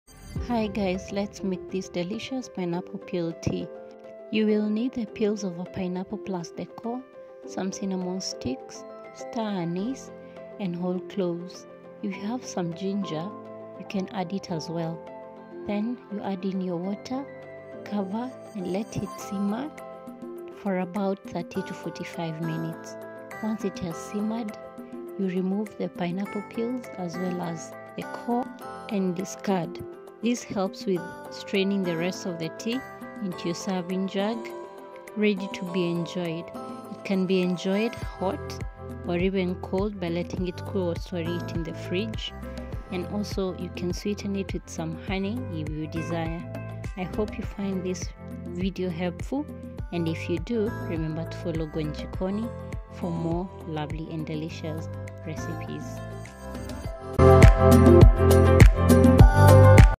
THE BEST ANTI-INFLAMMATORY TEA || How to make Pineapple Peel Tea || How to Recipes
This is as anti-inflammatory tea. Don't waste the pineapple peel anymore. Use the entire pineapple and make pineapple peel tea!

Here is how I love making mine and sometimes I add in ginger, turmeric and black pepper.

Artist: @tellyourstorymusicbyikson